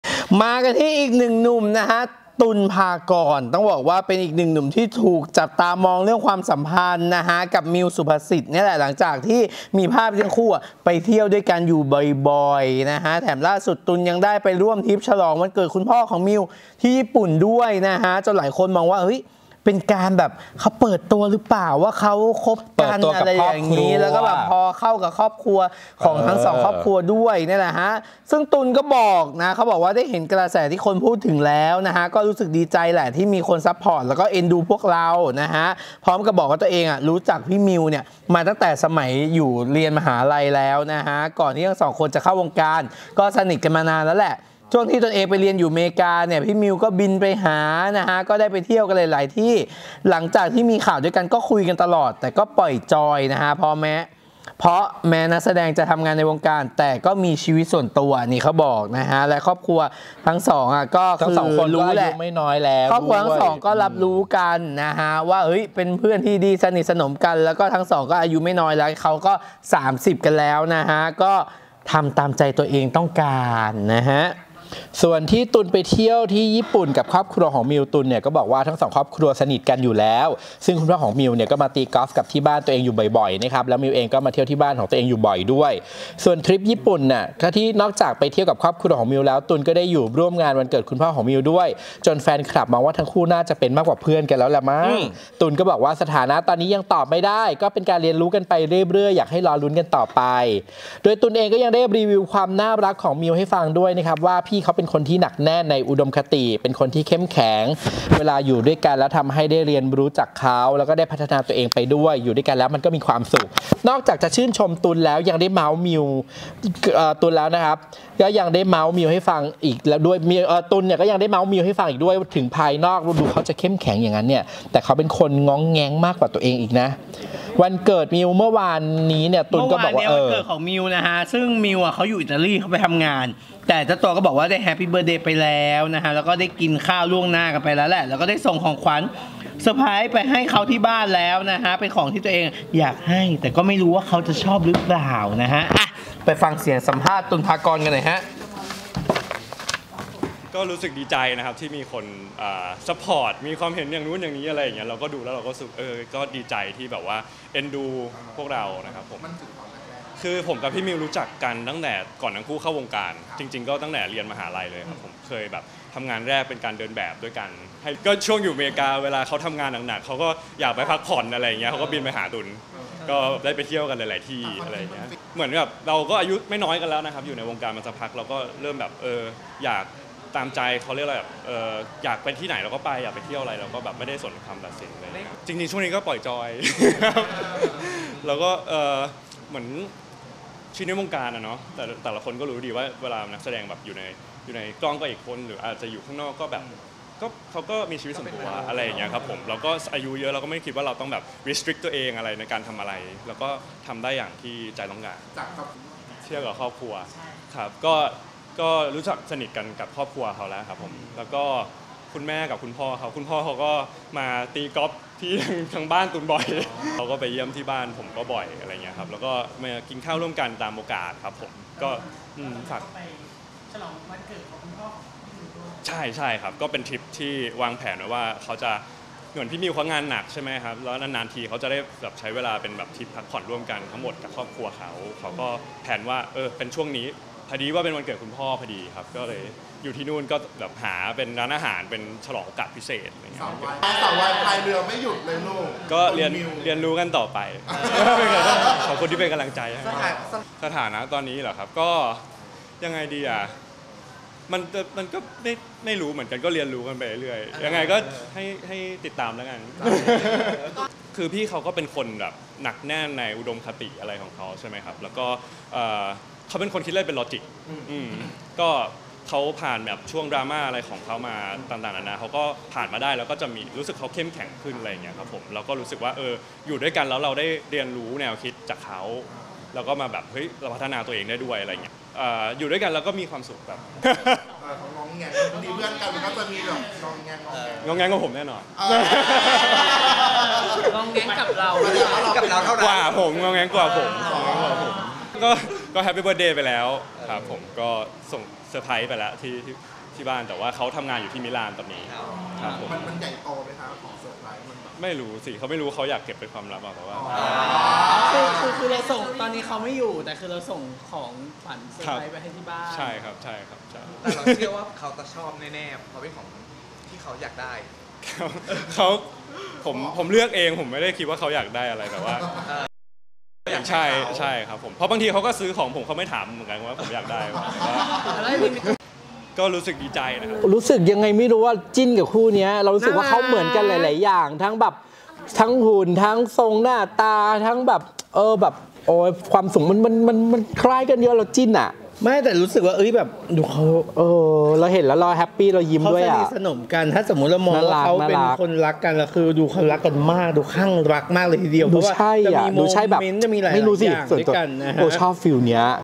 ‘ตุลย์ ภากร’ แง้มสถานะ ‘มิว ศุภศิษฐ์’ เป็นการเรียนรู้กันไปเรื่อยๆ ยอมรับอยู่ด้วยแล้วมีความสุข
‘ตุลย์ ภากร’ แง้มสถานะกับ ‘มิว ศุภศิษฐ์’ ตอนนี้ยังตอบไม่ได้ เป็นการเรียนรู้กันไปเรื่อย ๆ แต่ยอมรับอยู่ด้วยกันแล้วมีความสุข

หนุ่ม ‘ตุลย์ ภากร’ ที่ถูกจับตามองเรื่องความสัมพันธ์กับ ‘มิว ศุภศิษฎ์’ หลังจากมีภาพที่ทั้งคู่ไปเที่ยวด้วยกันบ่อย แถมล่าสุด ‘ตุลย์’ ยังได้ไปร่วมทริปฉลองวันเกิดคุณพ่อของมิวที่ญี่ปุ่นด้วย จนหลายคนมองว่าเป็นการเปิดตัวกับครอบครัวของมิว

ซึ่ง ‘ตุลย์’ ก็เปิดใจว่า ได้เห็นกระแสที่คนพูดถึงแล้ว ก็รู้สึกดีใจที่มีคนซัพพอร์ตและเอ็นดูพวกเรา พร้อมเผยว่ารู้จักกับ ‘มิว’ มาตั้งแต่สมัยมหาวิทยาลัย ก่อนที่ทั้งสองคนจะเข้าวงการ ก็สนิทกันมานานแล้ว ช่วงที่ตนเองเรียนอยู่อเมริกา มิวก็บินไปหา ก็ได้ไปเที่ยวกันหลายๆ ที่ หลังจากที่มีข่าวด้วยกัน ก็คุยกันตลอด แต่ก็ปล่อยจอย เพราะแม้นักแสดงจะทำงานในวงการ แต่ก็มีชีวิตส่วนตัว และทั้งสองคนก็อายุไม่น้อยกันแล้ว ก็เลยทำตามที่ใจตัวเองต้องการกัน

ส่วนที่ ‘ตุลย์’ ไปเที่ยวญี่ปุ่นกับครอบครัวมิว ‘ตุลย์’ ก็บอกว่าทั้งสองครอบครัวสนิทกันอยู่แล้ว ซึ่งคุณพ่อของมิวก็มาตีกอล์ฟกับที่บ้านตนเองบ่อย และมิวก็มาเที่ยวที่บ้านตนเองบ่อยด้วย ส่วนทริปญี่ปุ่นที่ผ่านมา นอกจากไปเที่ยวกับครอบครัวของมิวแล้ว ตุลย์ก็ได้อยู่ร่วมงานวันเกิดคุณพ่อของมิวด้วย จนแฟนคลับมองว่าทั้งคู่น่าจะเป็นมากกว่าเพื่อนกันแล้ว ‘ตุลย์’ ก็บอกว่า สถานะตอนนี้ ยังตอบไม่ได้ ก็เป็นการเรียนรู้กันไปเรื่อยๆ อยากให้รอลุ้นกันต่อไป

โดย ‘ตุลย์’ ยังได้รีวิวความน่ารักของ ‘มิว’ ให้ฟังด้วยว่า พี่เขาเป็นคนที่หนักแน่นในอุดมคติ เป็นคนที่เข้มแข็ง เวลาอยู่ด้วยกันก็ทำให้ได้เรียนรู้จากเขา และได้พัฒนาตัวเองไปด้วย อยู่ด้วยกันก็มีความสุข ซึ่งนอกจากจะชื่นชมแล้ว ‘ตุลย์’ ยังได้เม้าท์ ‘มิว’ ให้ฟังอีกว่า ถึงภายนอกเขาจะเข้มแข็ง แต่เขาเป็นคนง้องแง้งมากกว่าตนเอง

ส่วนวันเกิดมิวเมื่อวานนี้ ‘ตุลย์’ ก็บอกว่าตอนนี้มิวไปทำงานที่อิตาลี แต่ก็ได้แฮปปี้เบิร์ธเดย์ไปแล้ว และได้ไปกินข้าวล่วงหน้ากันมาแล้ว และตนเองก็ได้ส่งของขวัญไปให้ที่บ้านเขาแล้ว เป็นของที่ตนเองอยากให้ แต่ก็ไม่รู้ว่าเขาชอบรึเปล่า